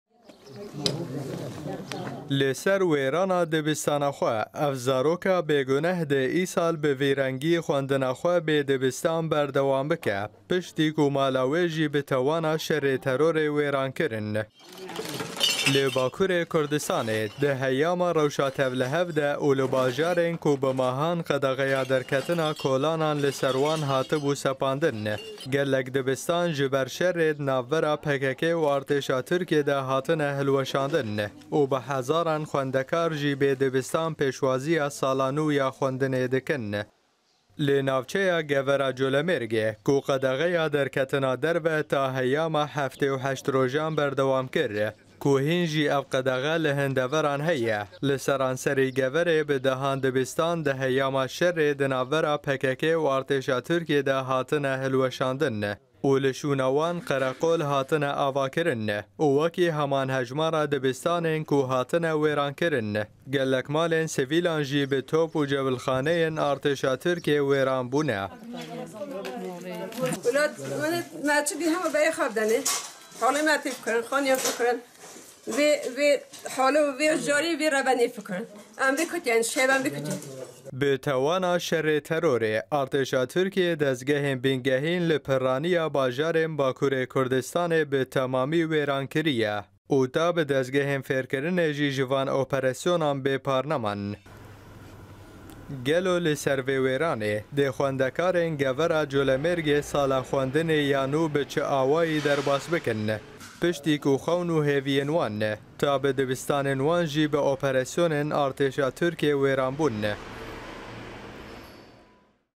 Xwendekar sala xwendinê ya nû li Geverê bê dibistan pêşwazî dikin
Li Bakurê Kurdistanê , di heyama rewşa tevlihev de ,xwendekarên bajarê Geverê yê bi ser Colemêrgê ve sala xwendinê ya nû bê dibistan pêşwazî dikin , piştî ku dibistan û malên wan jî di heyama qedxeya derketinê û şerê di navbera PKKê û Artêşa Tirkiyê de wêran bûn